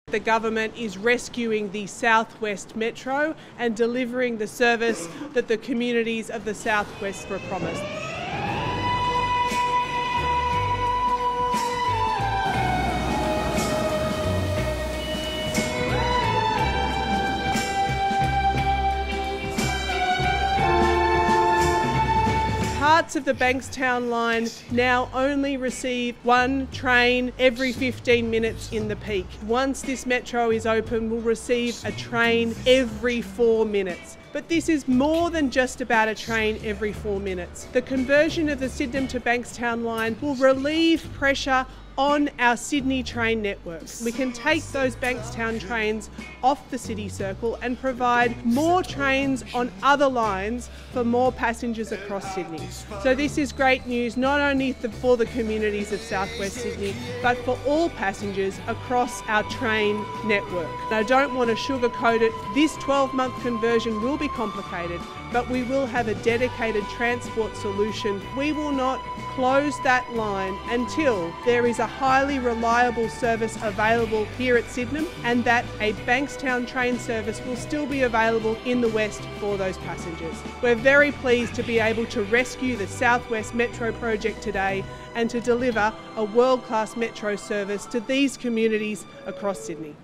We're delivering Metro Southwest
When we came into office, it was clear the Liberals grossly underestimated the complexity of this work, and hadn’t done the work to plan out how they’d deliver this project.

We’ve taken the time to plan this project properly.

We’ll be able to deliver metro services quicker, and ease the pain caused by converting the line over 12 months –instead of the 15-18 months sketched out by the previous Government.